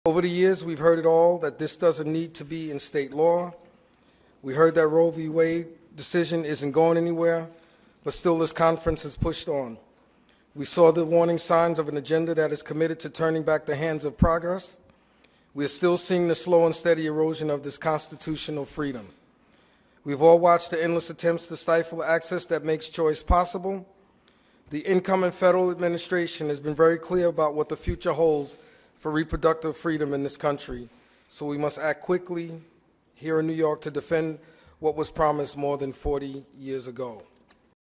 Women's Reproductive Rights Press Conference
NYSAssembly Speaker Carl E. Heastie and the NYSAssemblyMajority held a press conference highlighting the importance of standing with all New York women and turning meaningful women’s reproductive rights legislation into law. “Over the years, we have heard it all - that ‘This doesn’t need to be in state law.’ We hear that ‘The Roe v. Wave decision isn’t going anywhere.’ Still, this conference has pushed on. We saw the warning signs of an agenda that is committed to turning back the hands of progress. We are still seeing the slow and steady erosion of this constitutional freedom,” said Speaker Heastie.